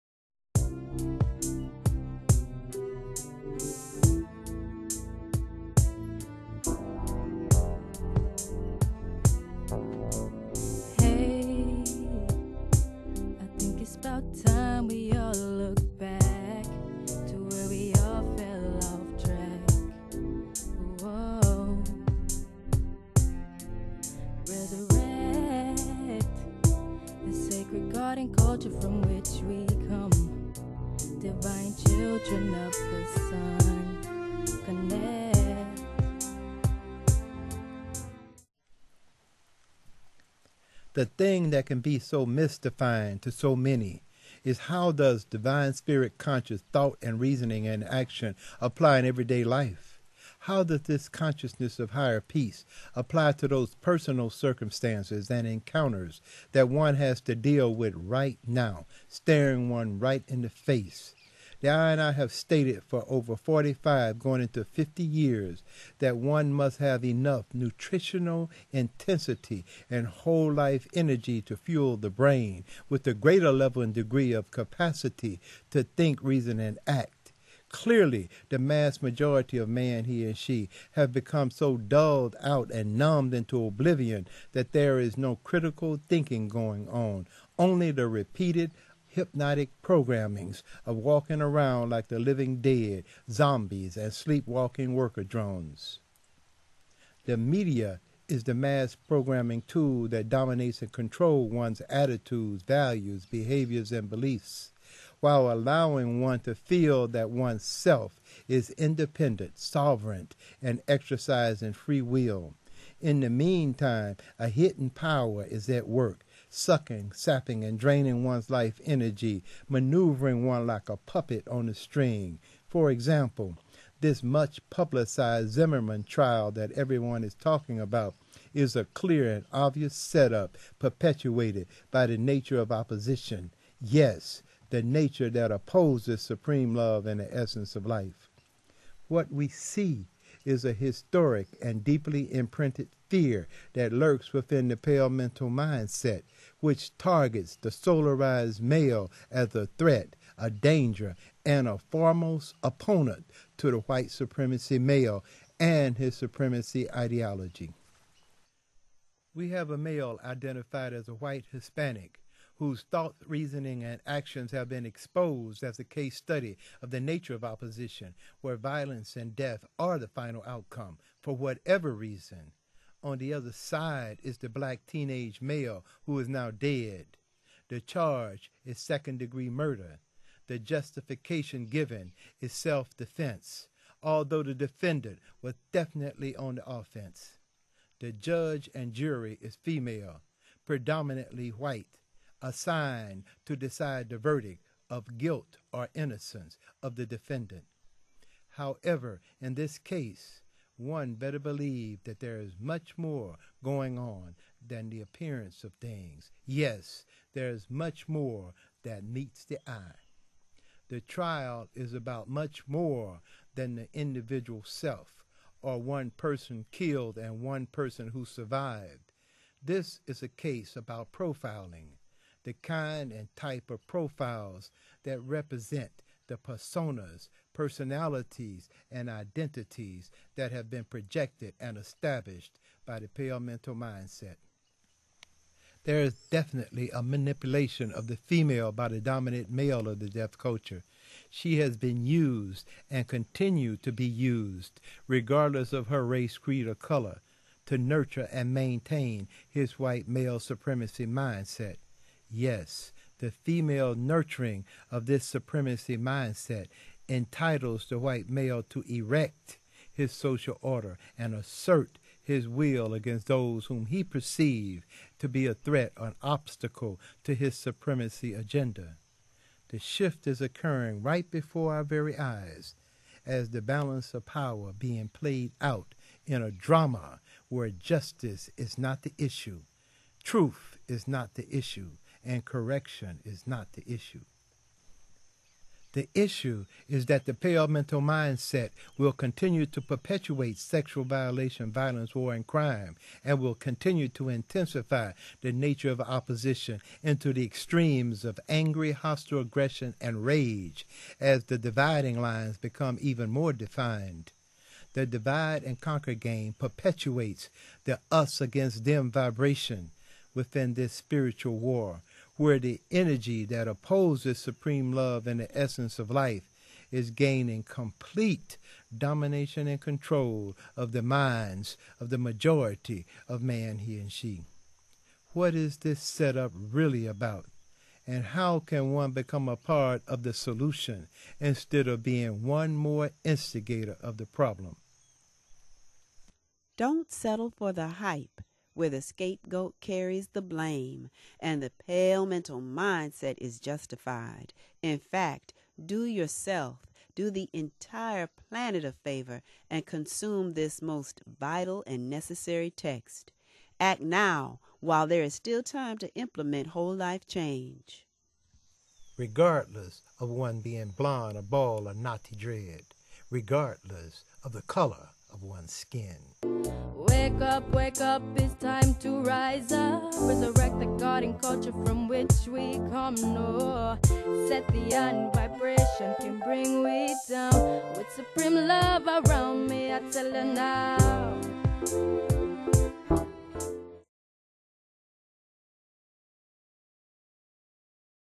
Molding the Generation Next: Noting the Zimmerman Trial and the Manipulation of Feminine Energy
For more sacred ancestral clarity, insight and inspiration get your copy of "Spiritual Analysis of Western Culture and the Re-Akwakening of Naga Consciousness.